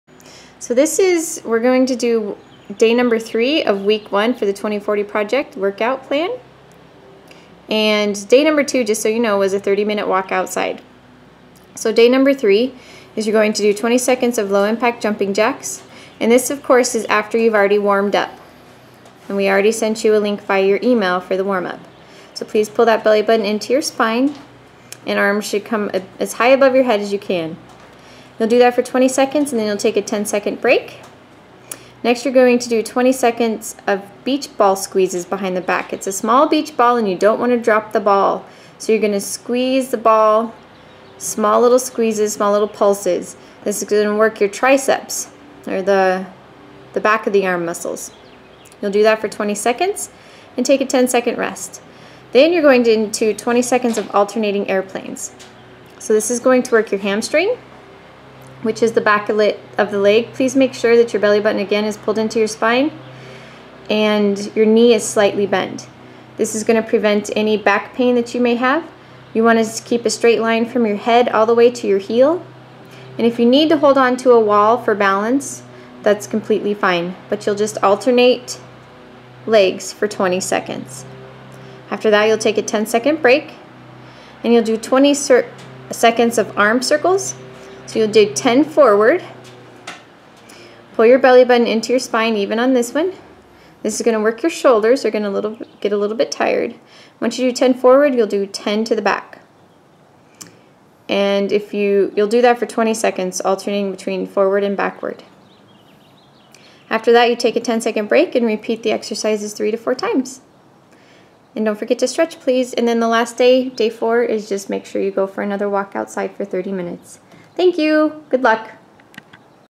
20/40 Project Week 1 Workout 2
Beginning workout for those wanting to start exercising without causing injury. Warm up for 10 minutes then do this workout through 3 to 4 times as fast as safely as possible.
The days between the workouts should consist of at least 30 minutes walking or swimming.
Consult your doctor before beginning any exercise regimen.